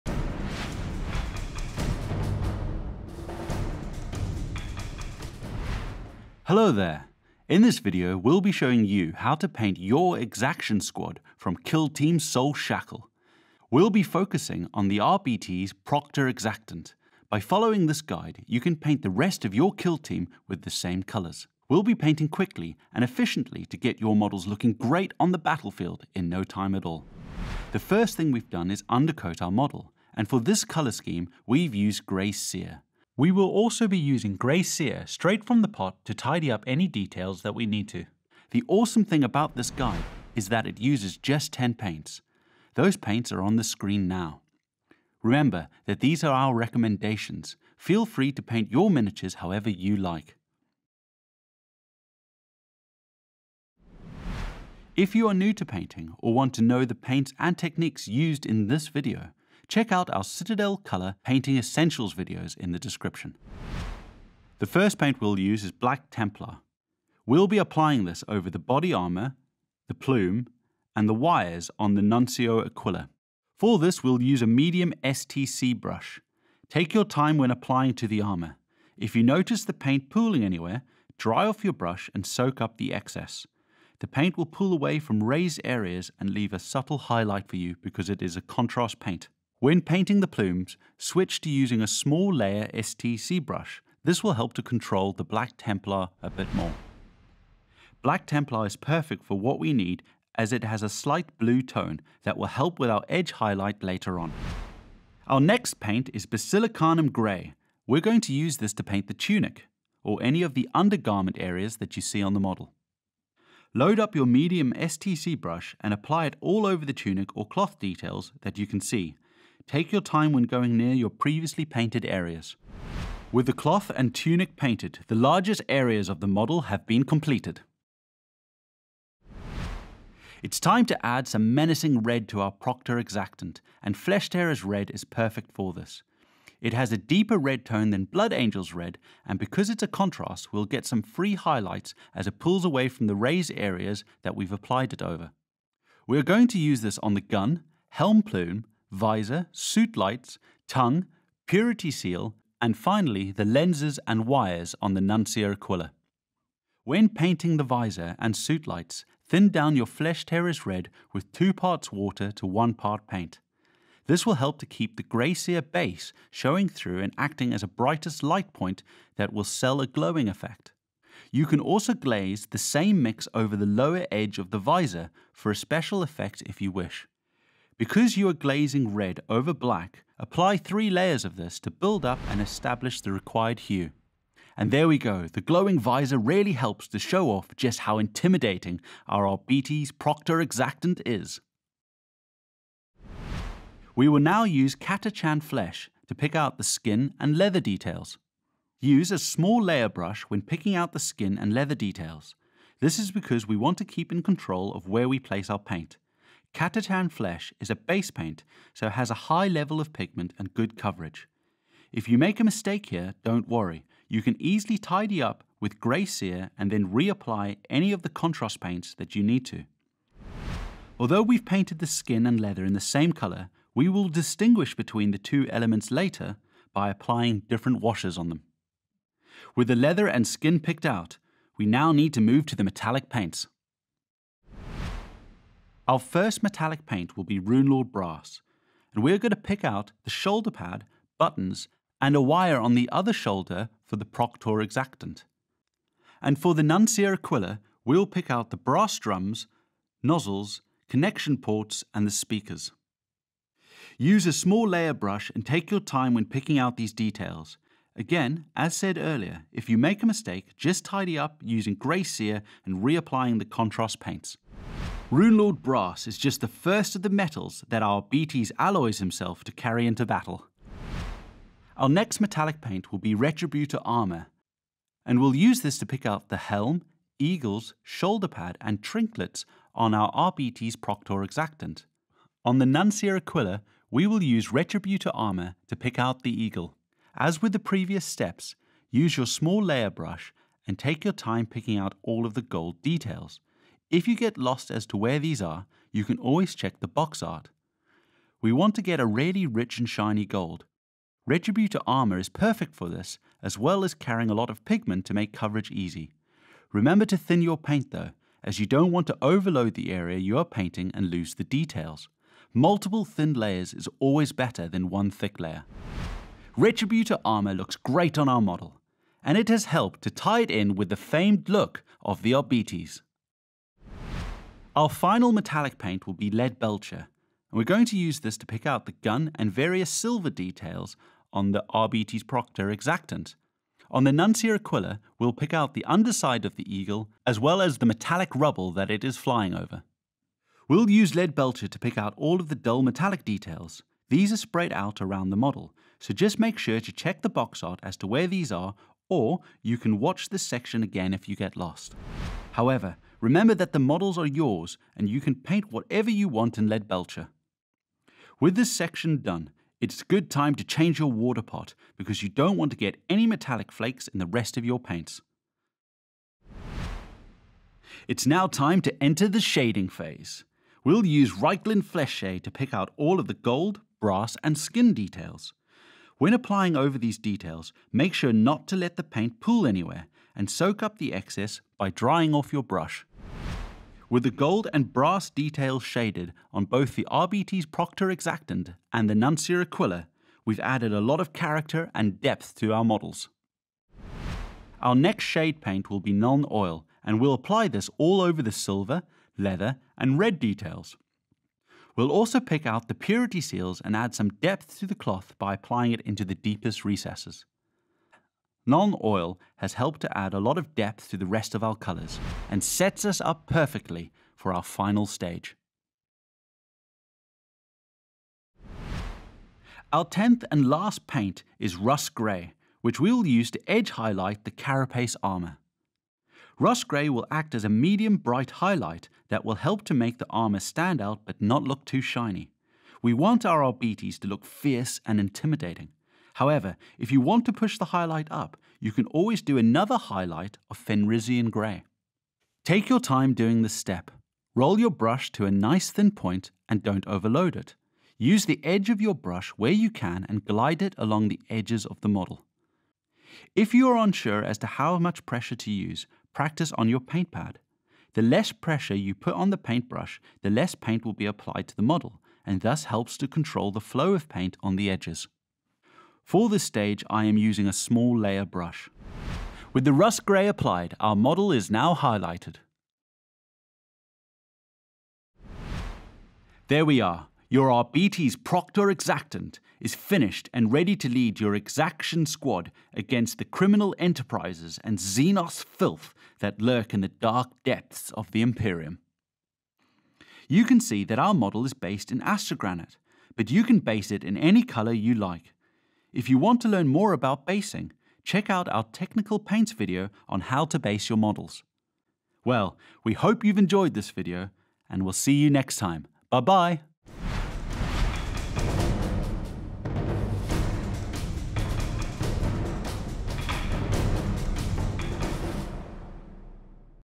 How to Paint: Kill Team - Exaction Squad - Arbites Proctor-exactant
Arbites Proctor-exactant's are veterans who are highly trained enforcers of Imperial justice.

Follow this step-by-step guide, to paint the Arbites Proctor-exactant in 10 paints, which you can also use on the entire squad, getting them set for games of Kill Team!

If you're new to painting - check out our Citadel Colour Painting Essentials videos to learn all about it